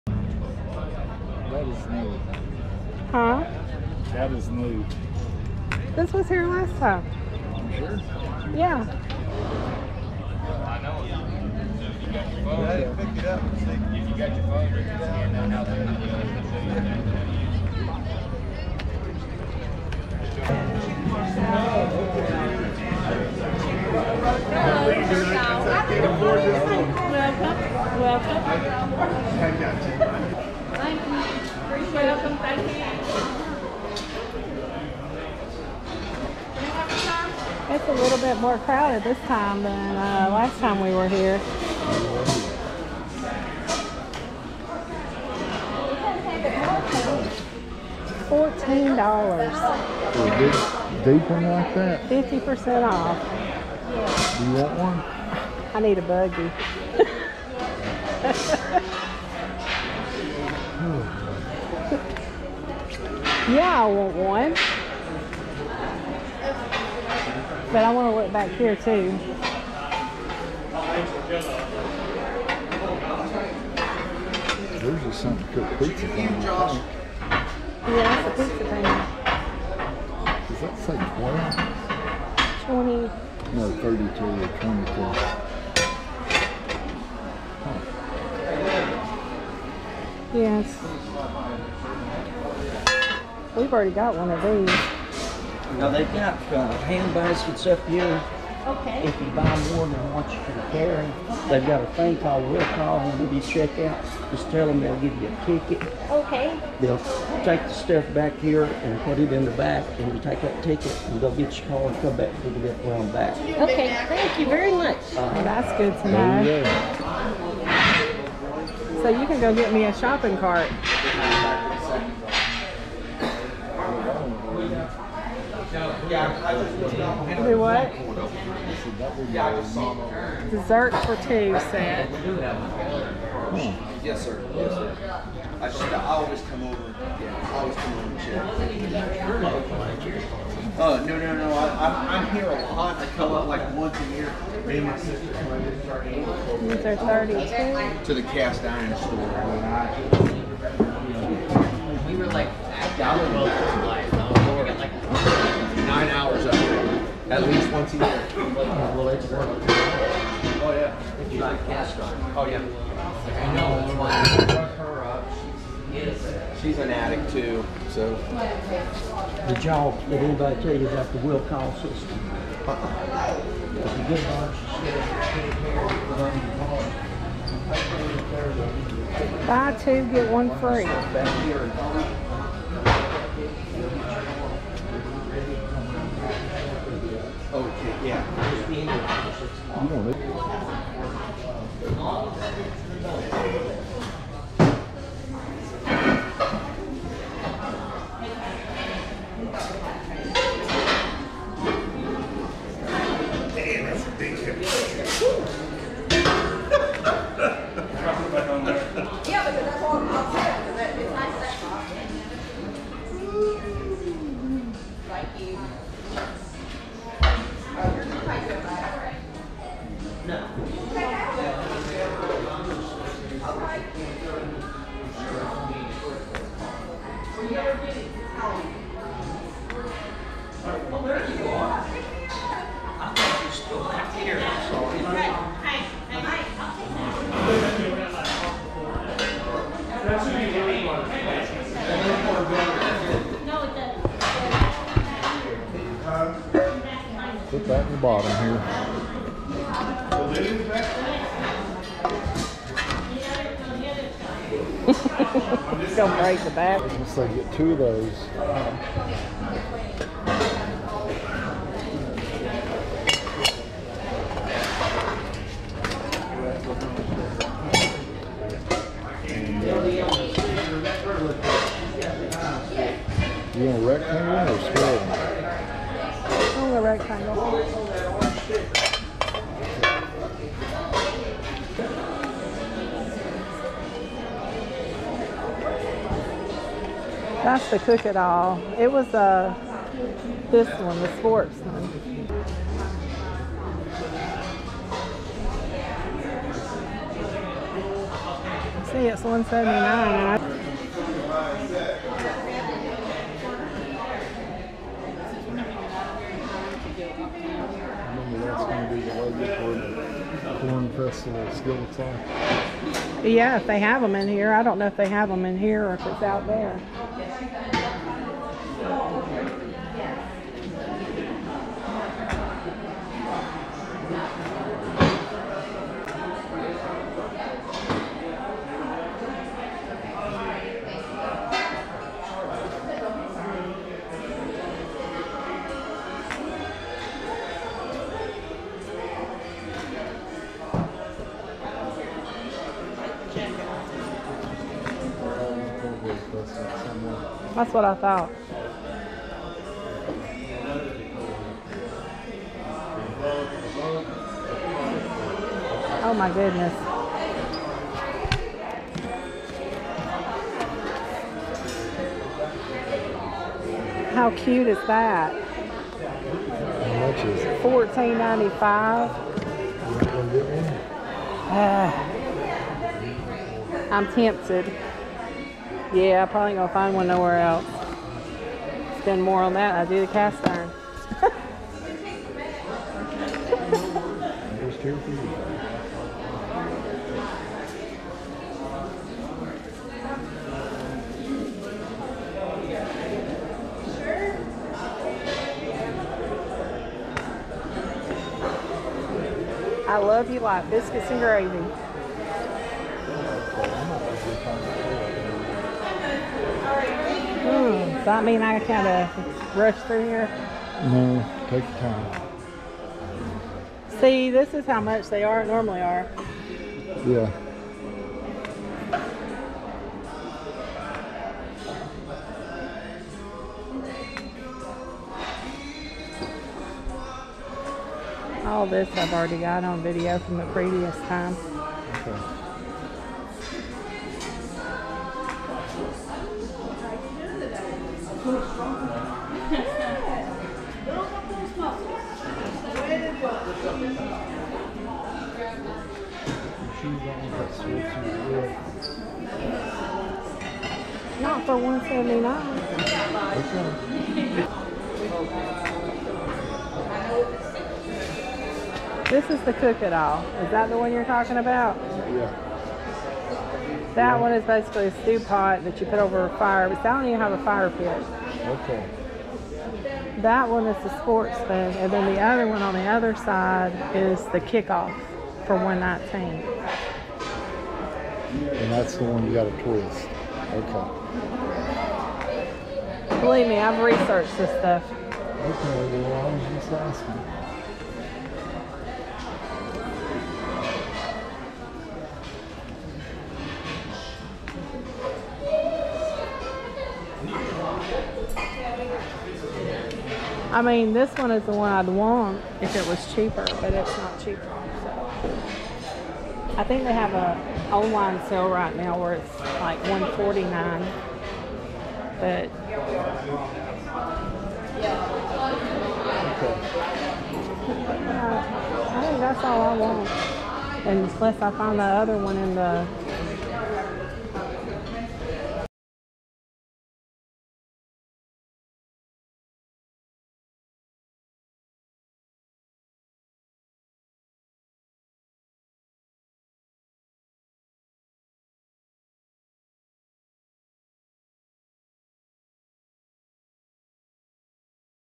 Shopping at the Lodge Factory Store - South Pittsburg, TN
Come shopping with us at the Lodge Factory Store in South Pittsburg, TN. We went here for the National Cornbread Festival and stopped in to see what deals that had on cast iron. I bought a few items.

For cooking videos check out @BobbiJo

***Send us a note in the mail***
Chastity & Kim